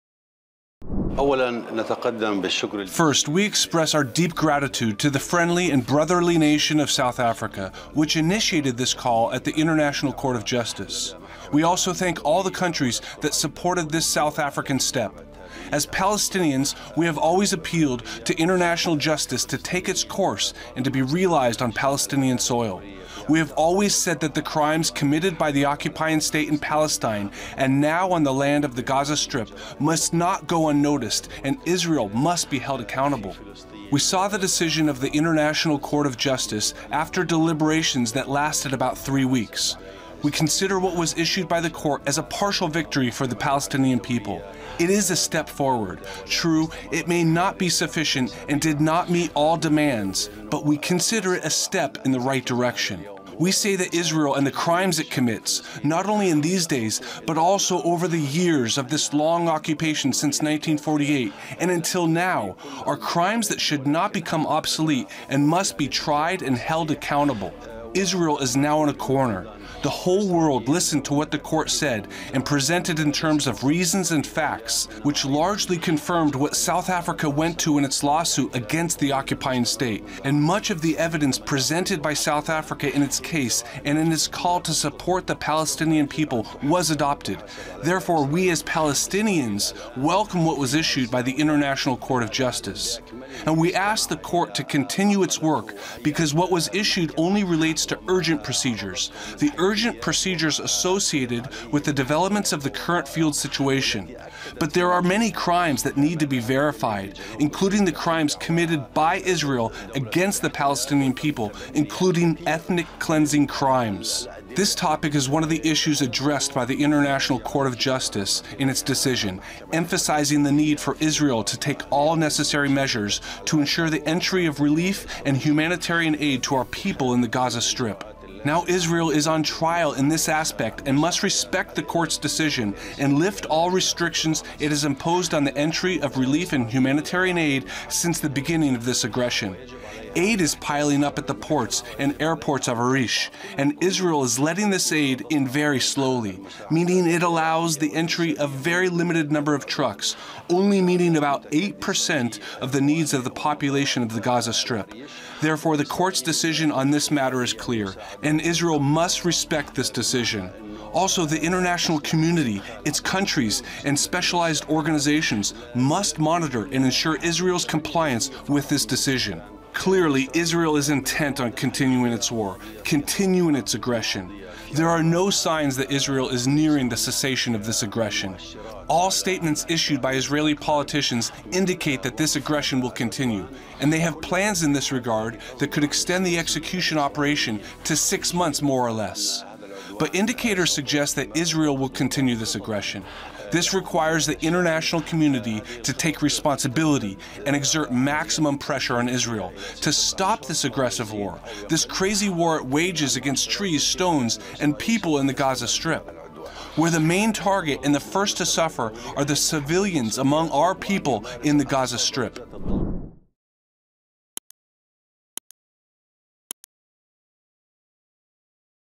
“Israel Must Be Held Accountable”: Palestinian Envoy To Turkiye | Dawn News English
Palestinian Ambassador to Turkey, Faed Mustafa, emphasized the imperative of holding Israel accountable in the aftermath of the International Court of Justice's (ICJ) judgment. Speaking to RT correspondent Melda Dogan, he conveyed the significance of seeking accountability for Israel's actions.